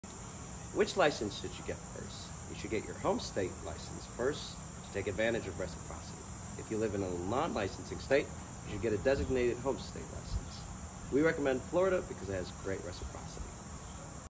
Which Adjuster License Should You Get First?
Our team member Vince answers, “Which insurance adjuster license should you get?”. Keep watching to hear the answer!